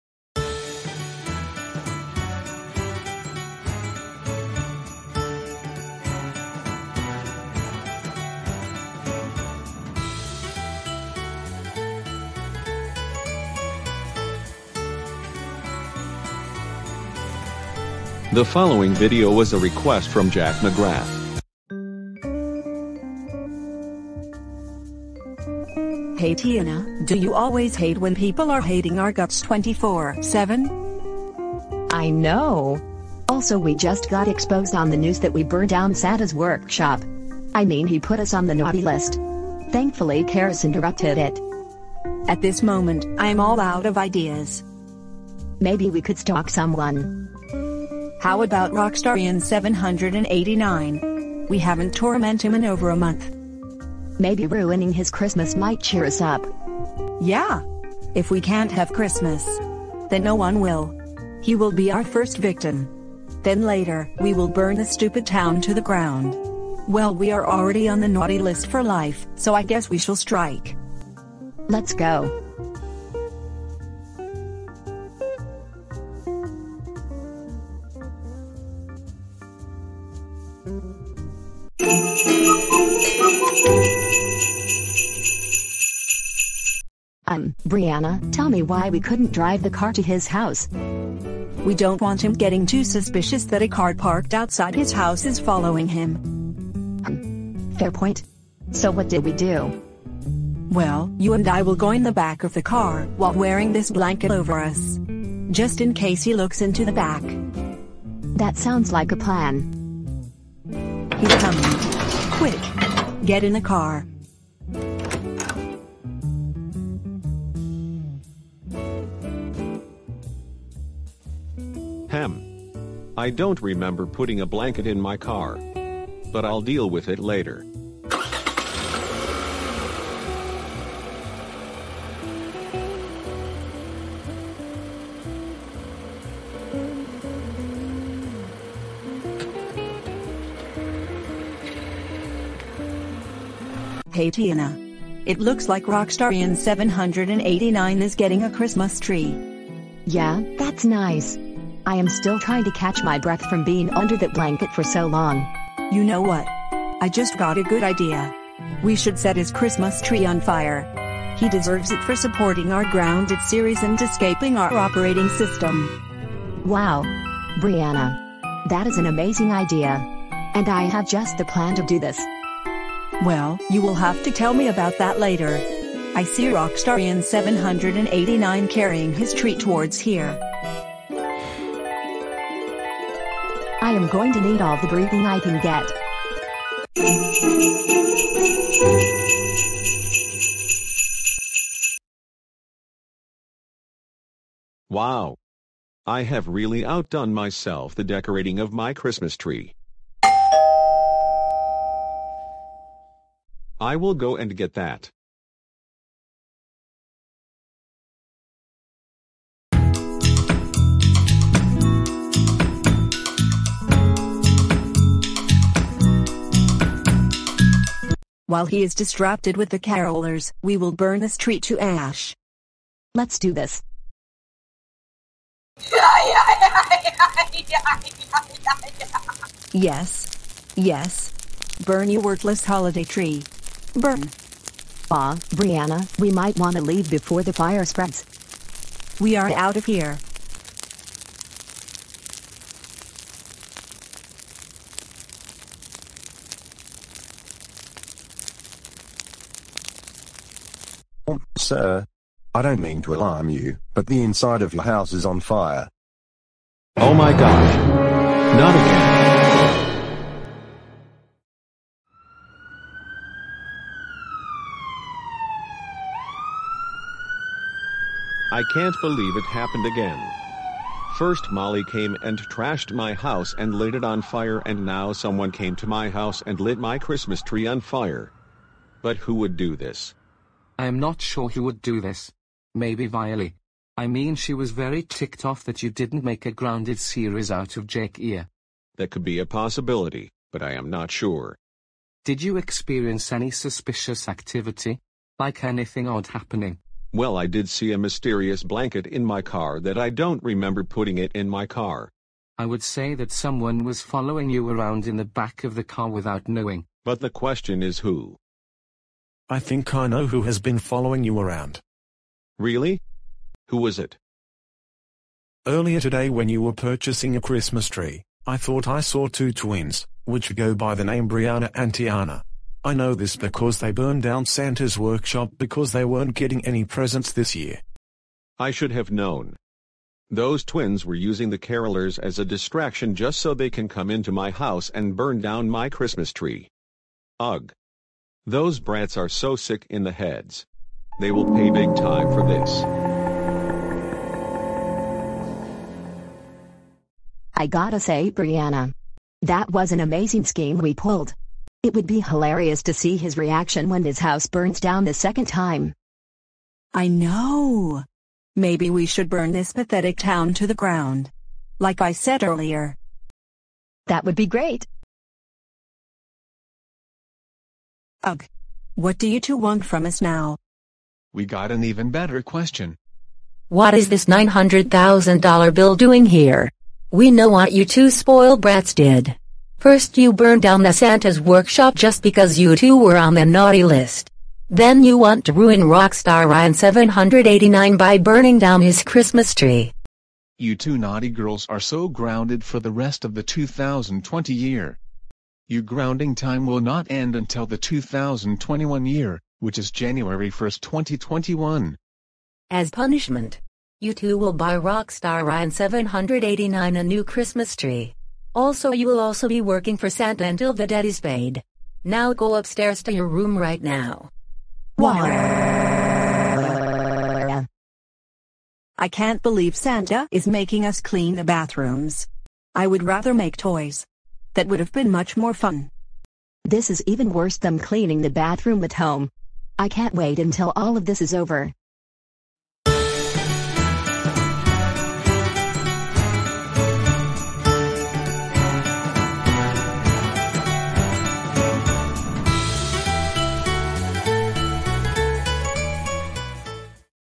Brianna and Tiana burns down my Christmas Tree 🎄 And Gets Grounded and Punished
Request by Jack McGrath

This is a skit for entertainment